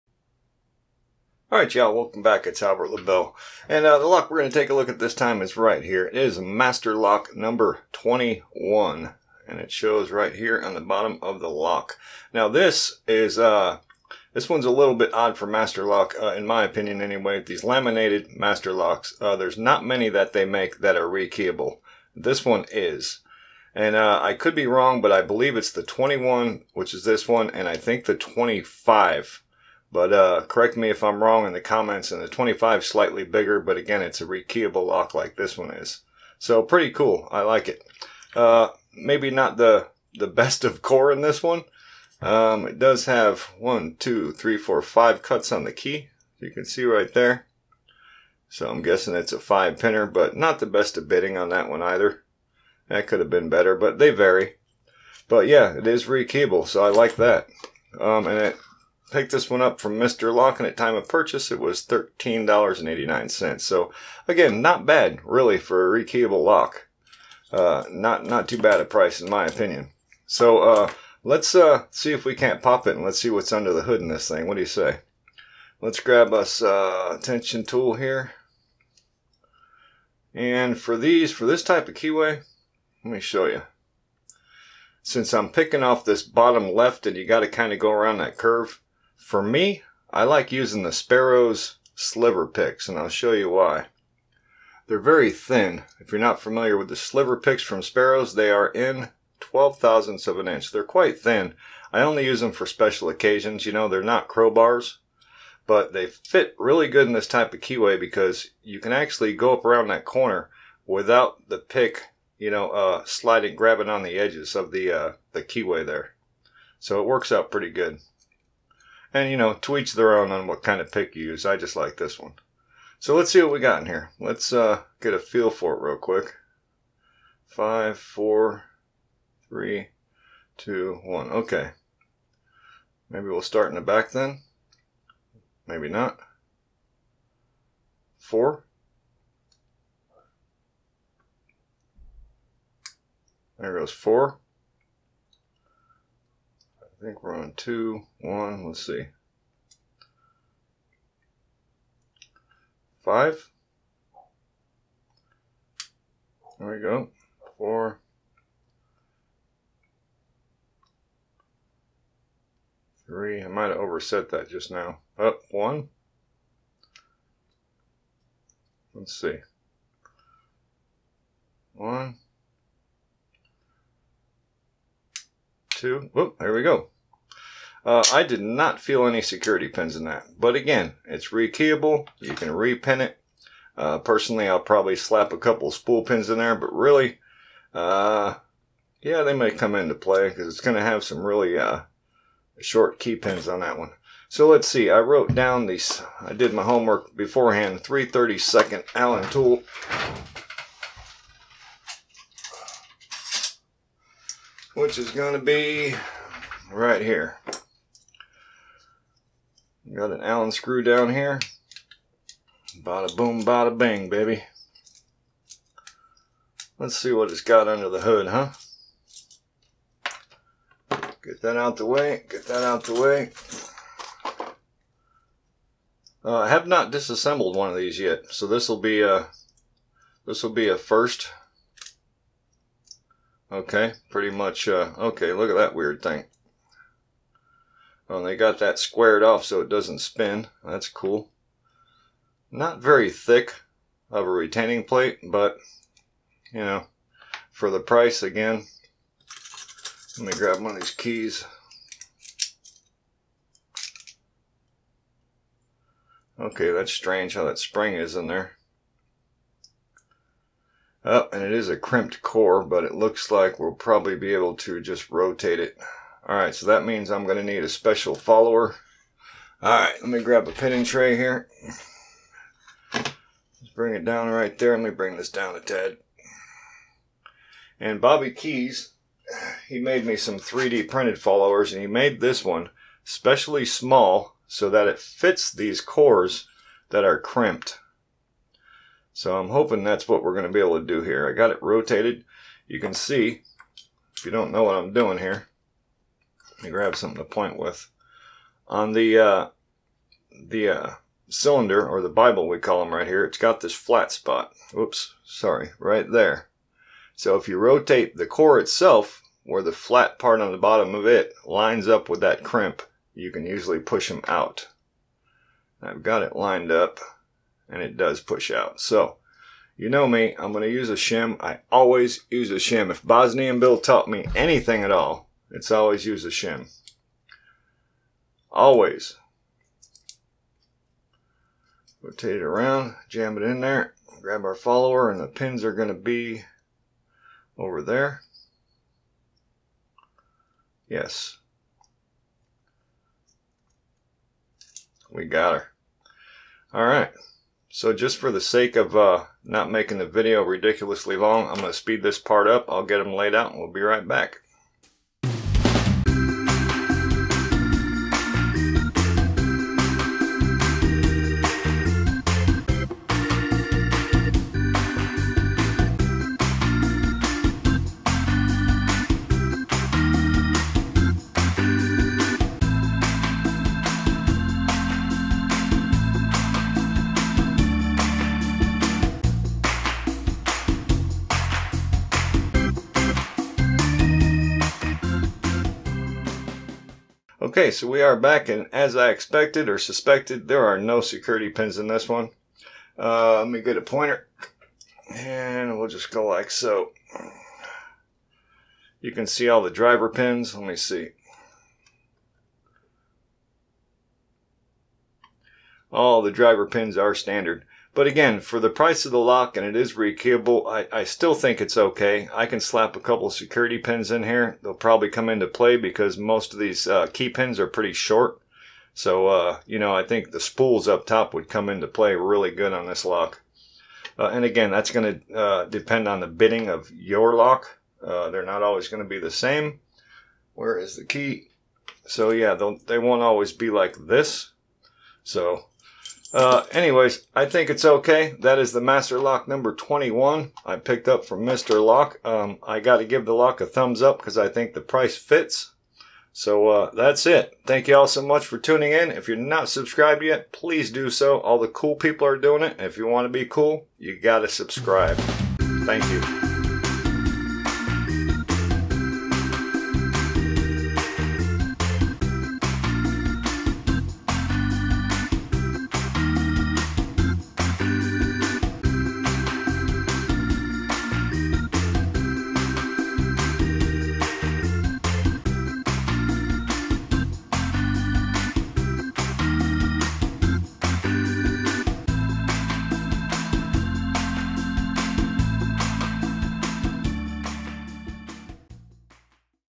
#418 Rekeyable master lock No 21
This master lock 21 is quite affordable for decent rekeyable padlock.
I always tell people any lock that can be disassembled can also be upgraded.
A couple security pins in the right spot and your all set.